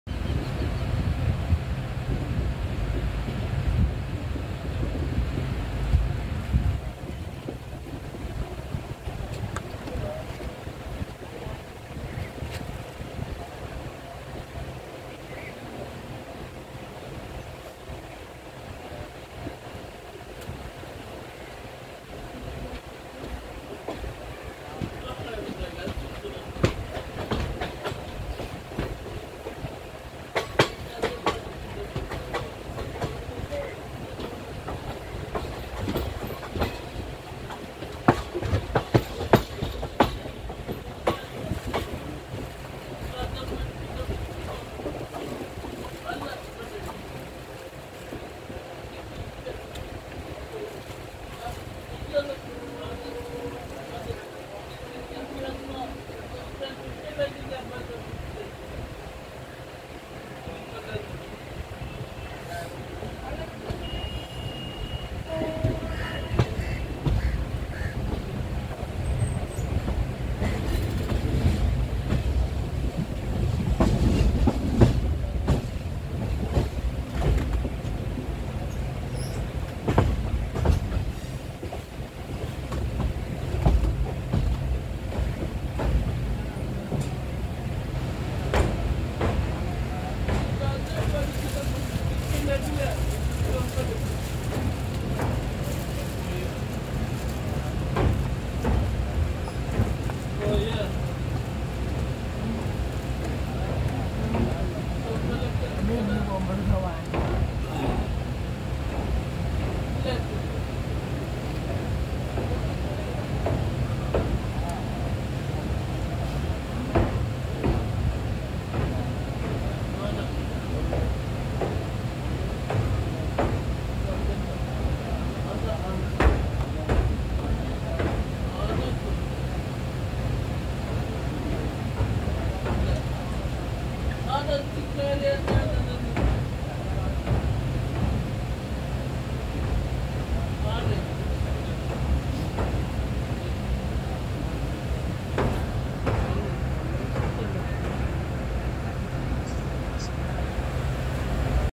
16306 Kannur Ernakulam Intercity Express Crossing Bharathapuzha
A great view from Kannur Ernakulam intercity Express crossing Bharathapuzha river.